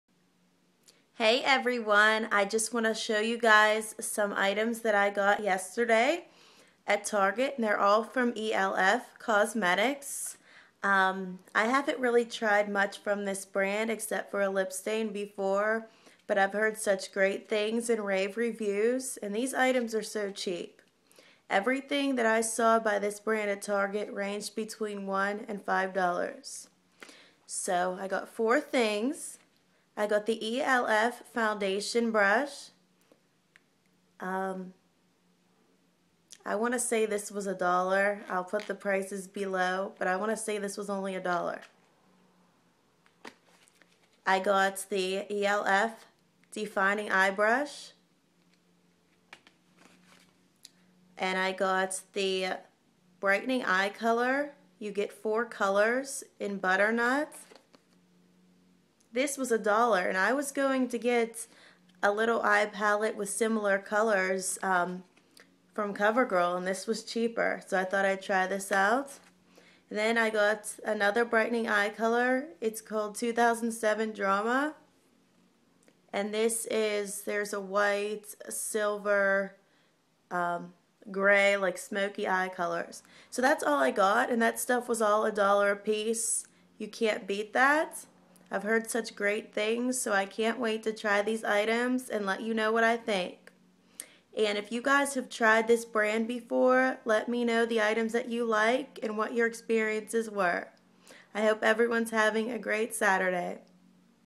ELF haul
Everything was $1!!

ELF Foundation Brush 1801
ELF Defining Eye Brush 1809
ELF Brightening Eye Color Butternut & Drama